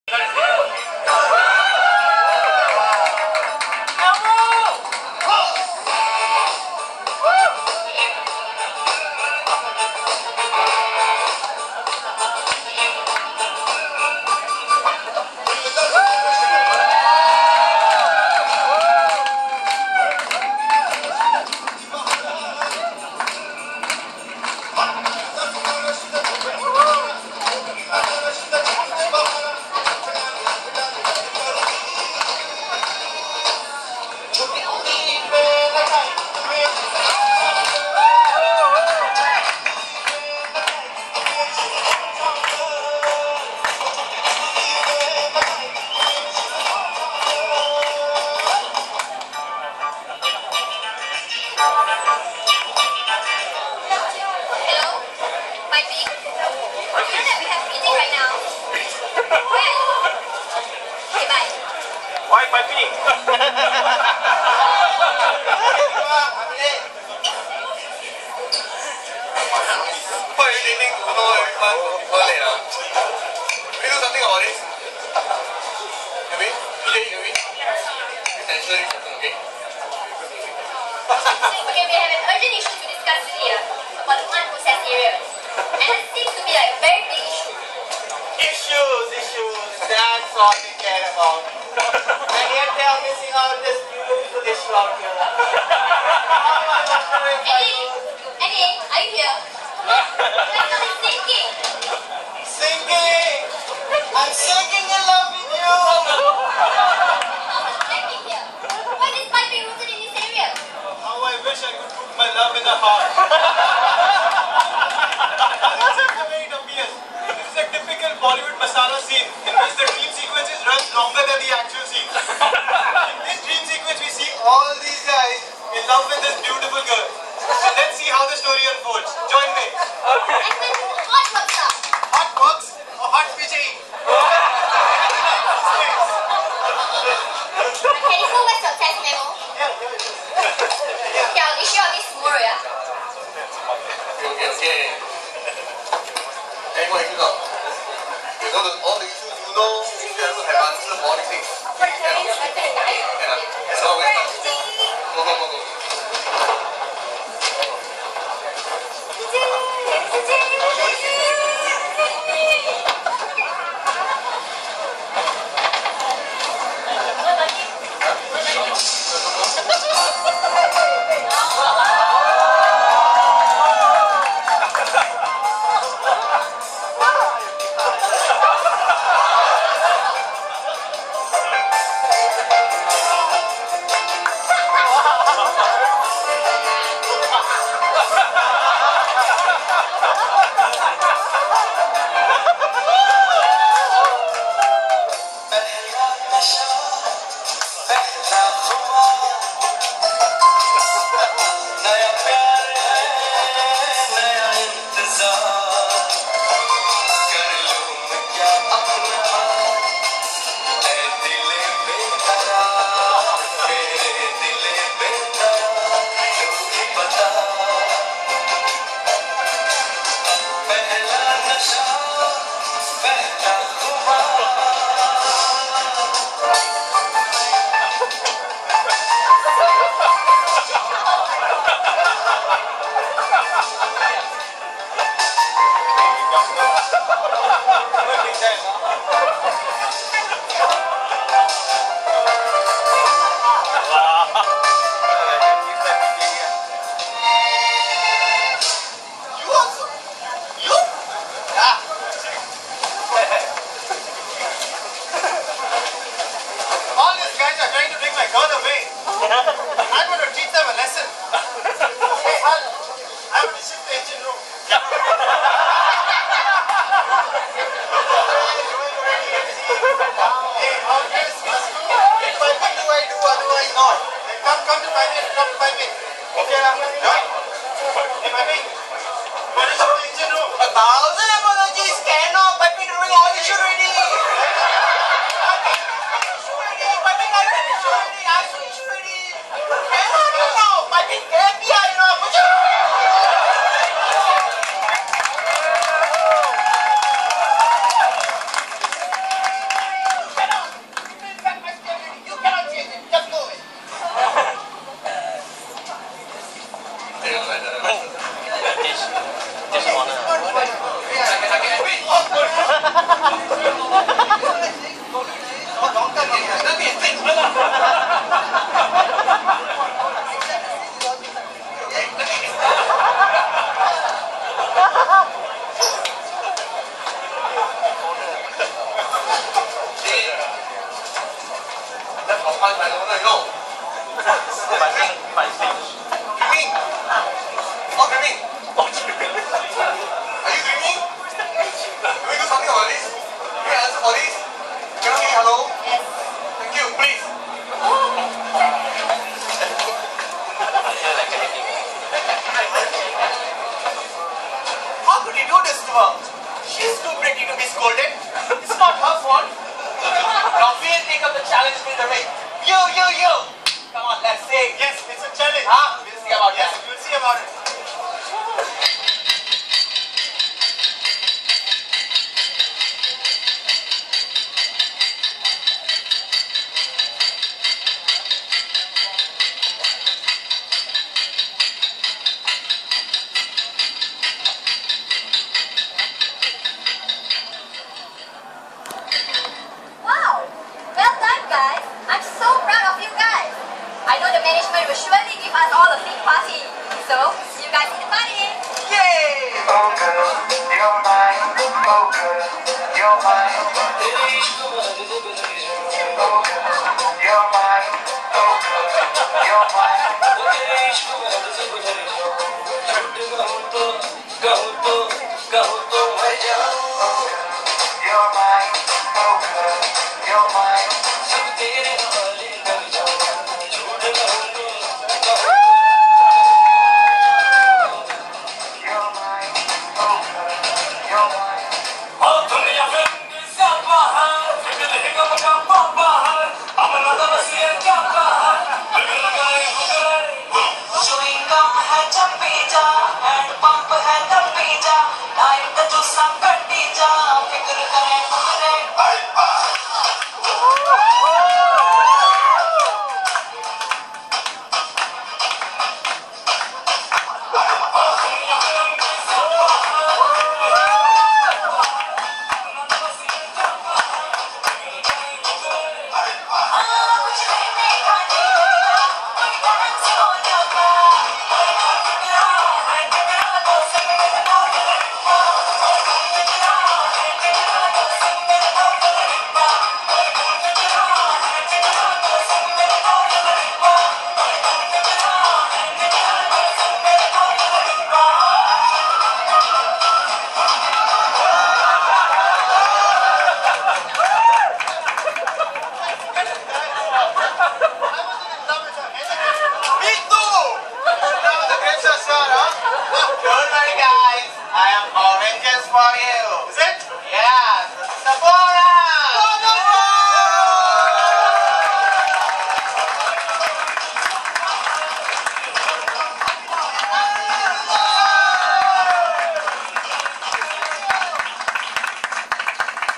Bollywood Masala 2010 EYP
Keppelfels End Year Party 2010,only 3weeks after i join.. i did this crazy thing infront of a few hundreds of engineers.